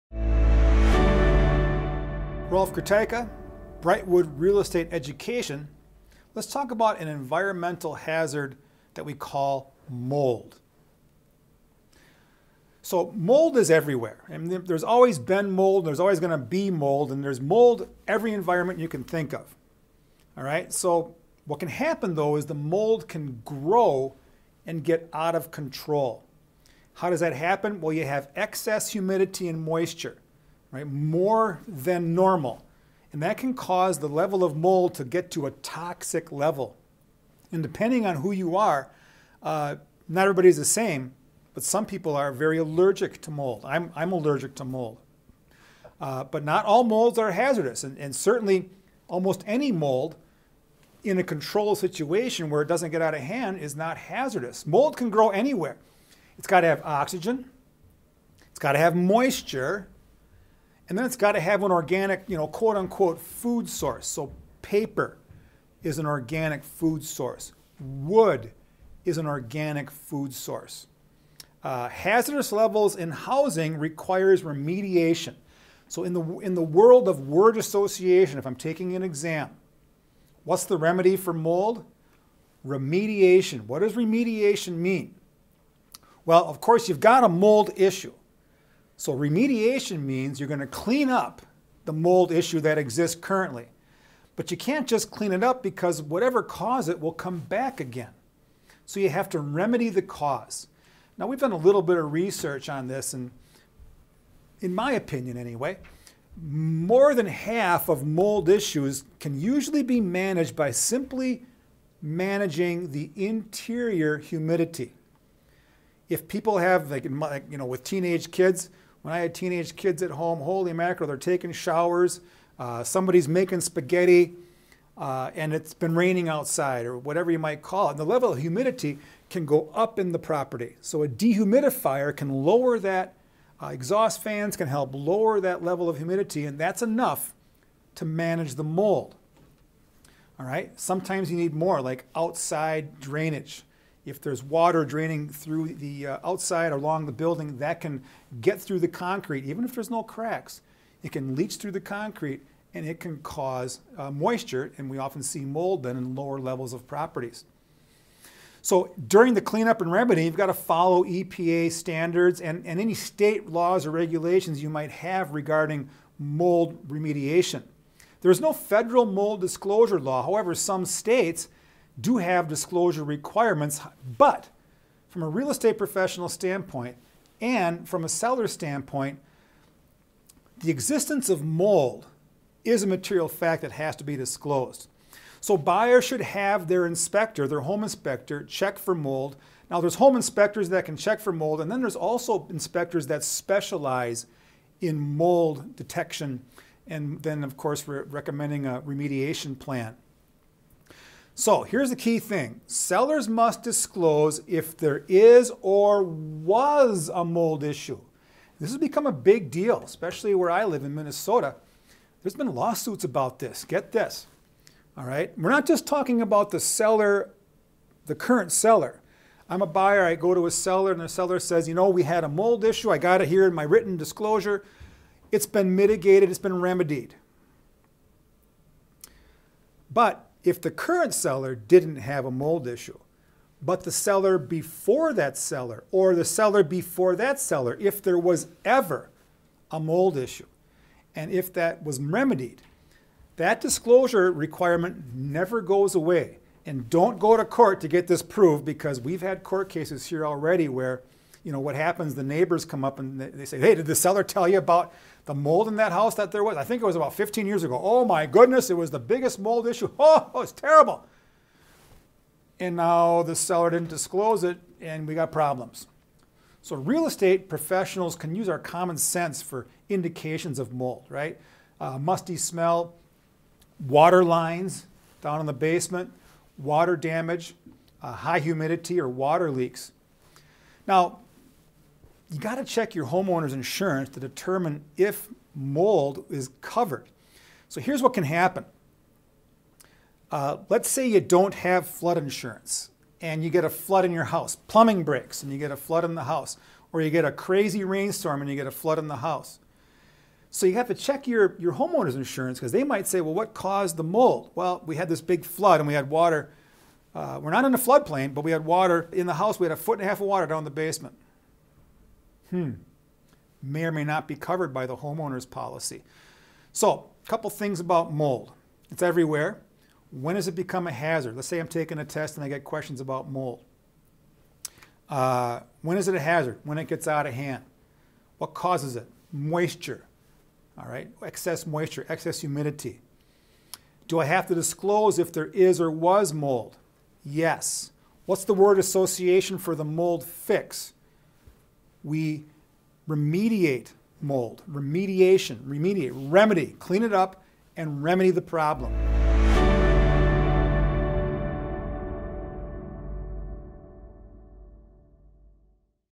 Real Estate Exam Prep: Mold
This video discusses mold, why it's a environmental hazard, causes, where it grows, remediation of mold, and disclosures from the sellers. Kaplan Real Estate Education offers live and online real estate licensing courses to help you pass your real estate exam.